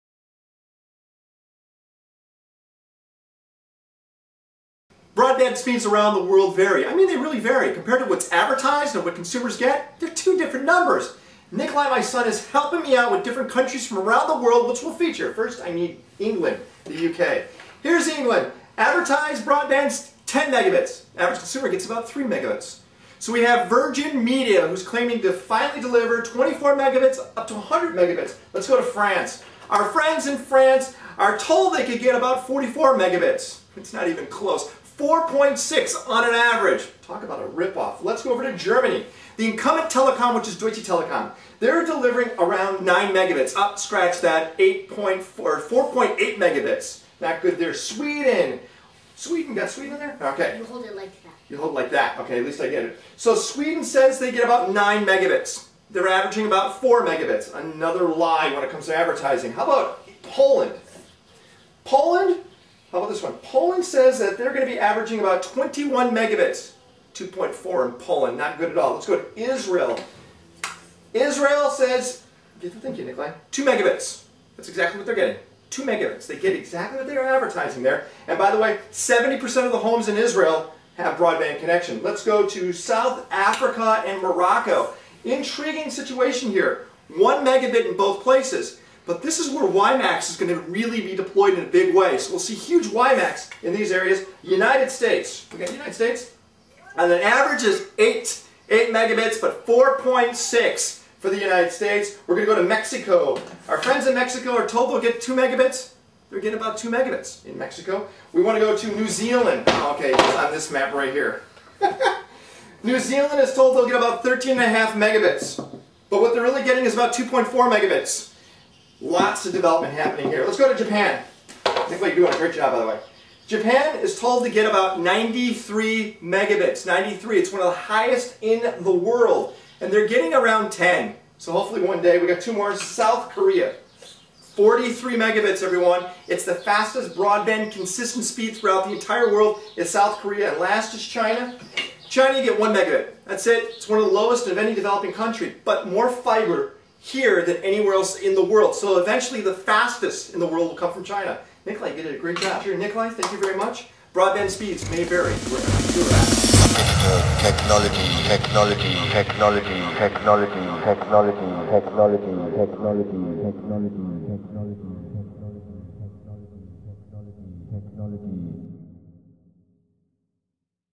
Global Broadband Speeds
Bandwidth providers across the global continue to mislead consumers when it comes to bandwidth. Speeds can be as much as 90% less an what is advertised. As consumer are offered more bandwidth alternatives, the incumbrance will need to guarantee the speeds being marketed.